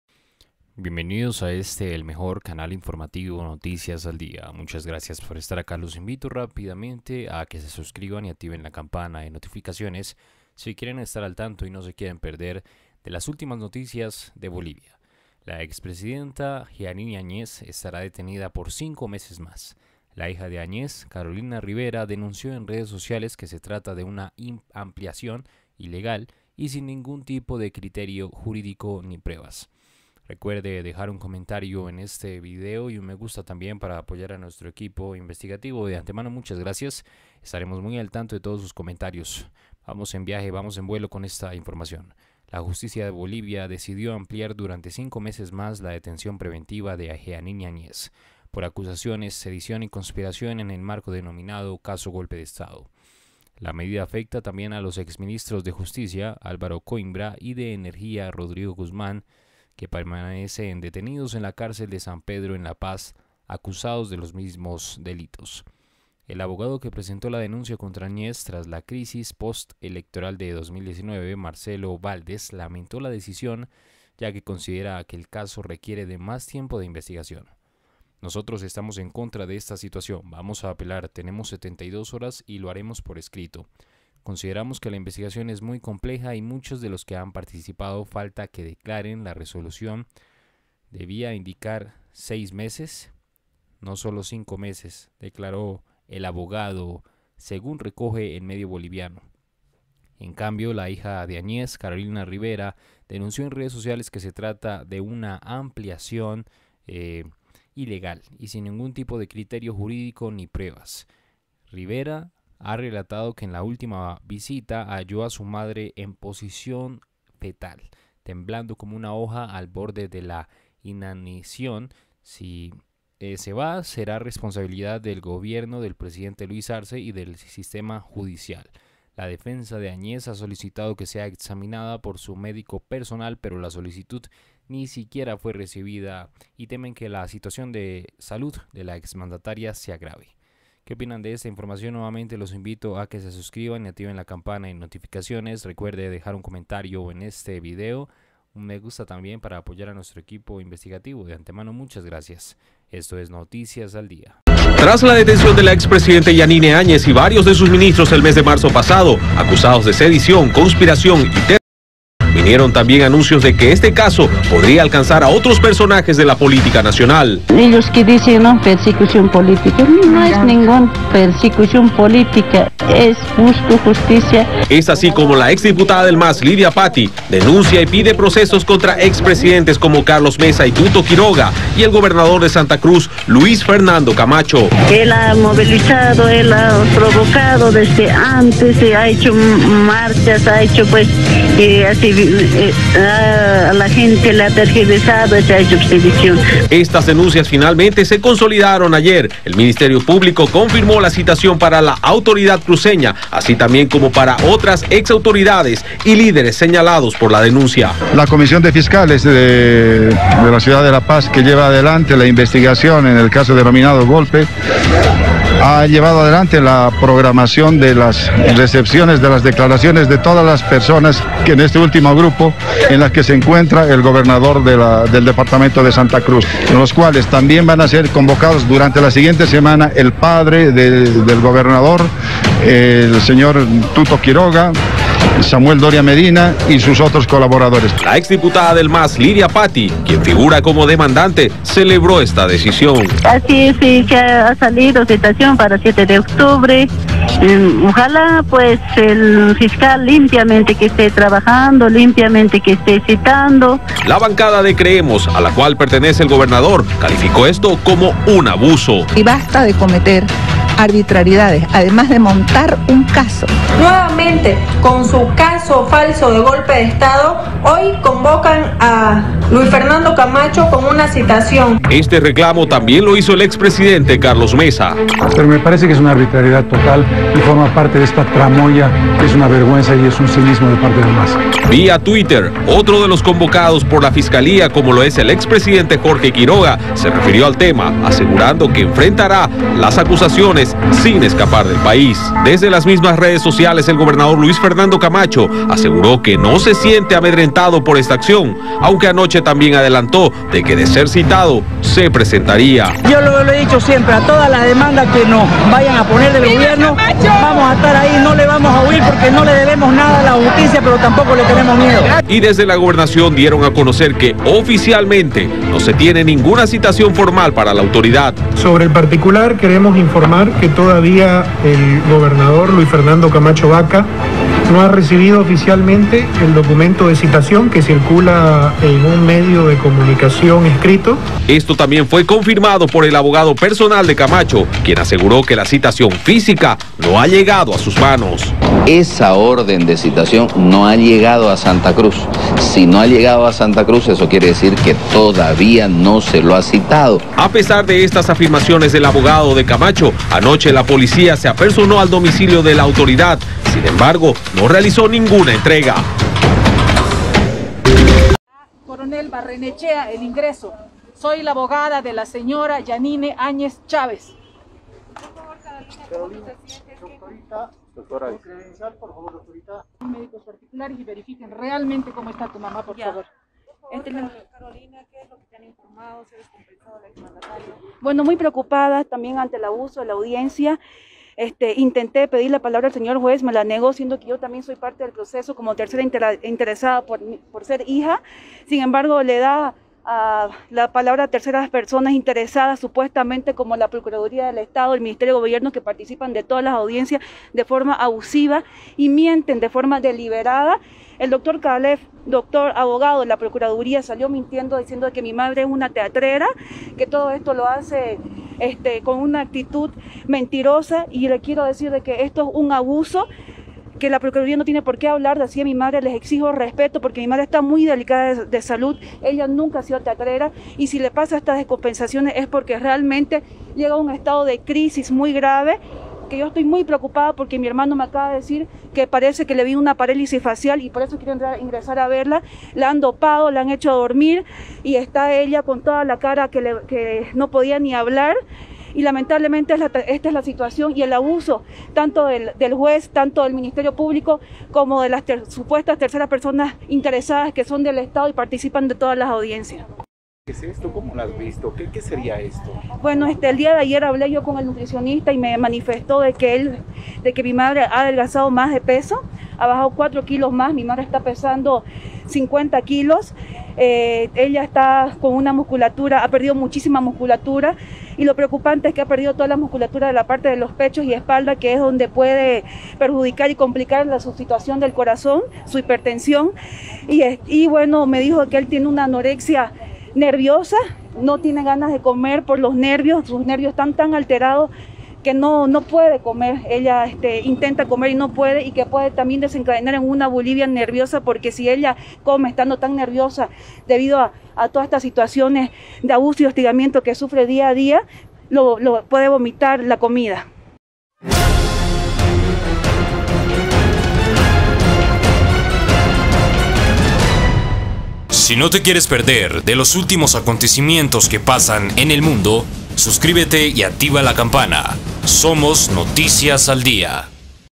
BOLIVIA HOY NOTICIAS 03 DE OCTUBRE 2021, ULTIMA HORA JEANINE AÑEZ PIDEN LIBERACION SEGUROS GAS HOY
Gracias!

ultima hora bolivia hoy 03 de octubre 2021
noticias de bolivia hoy 03 de octubre 2021